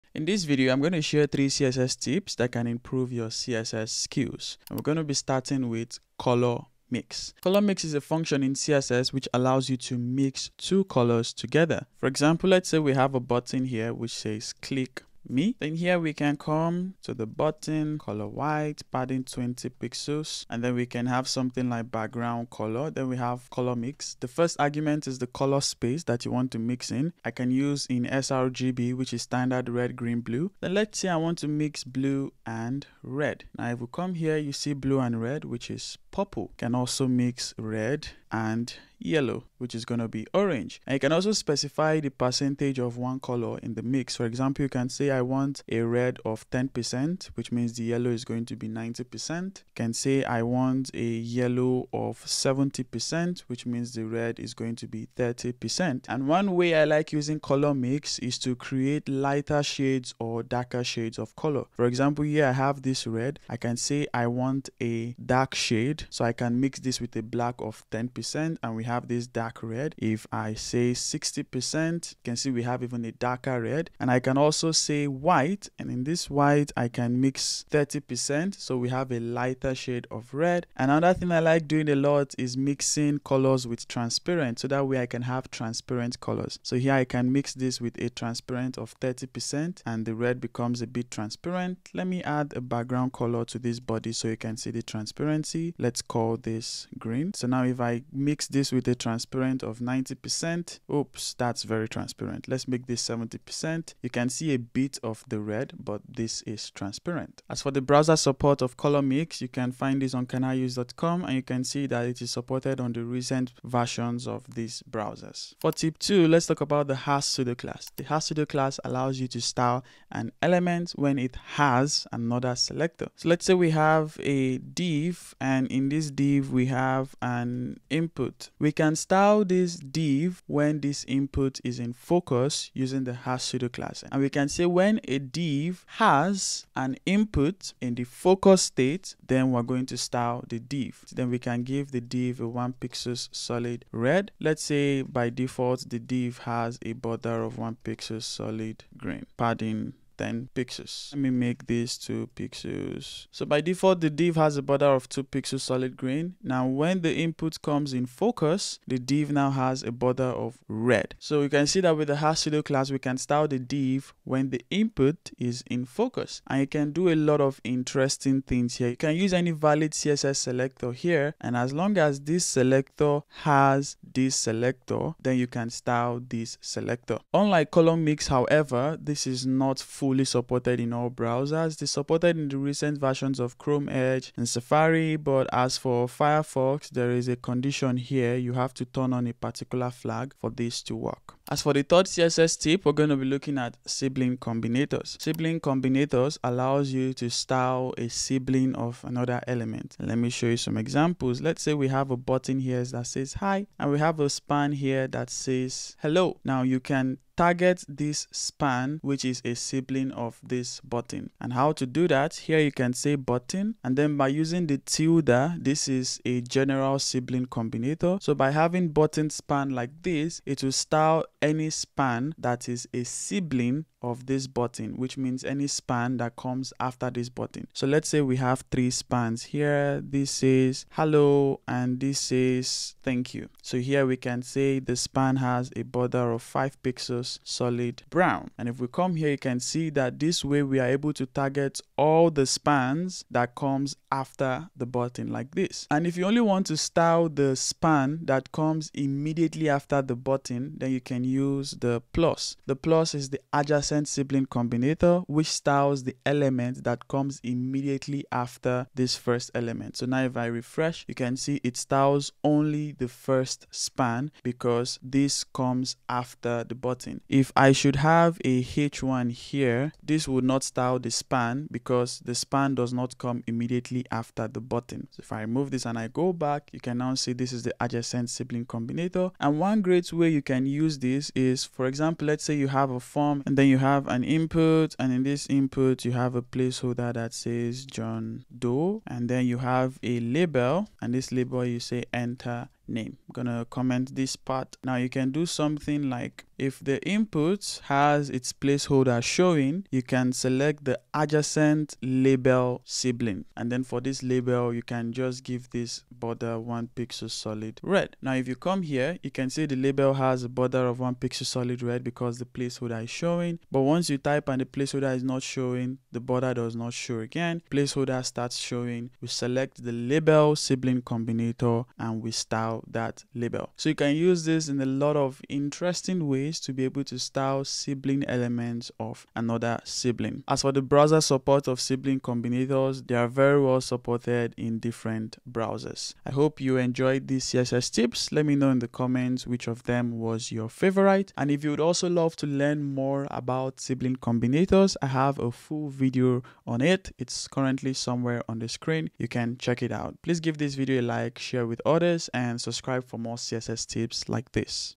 3 AMAZING CSS TIPS to make you a pro 🔥
Here are 3 CSS Tips that can improve your CSS development experience:

00:00 color-mix function
01:55 :has pseudo class
03:11 sibling combinators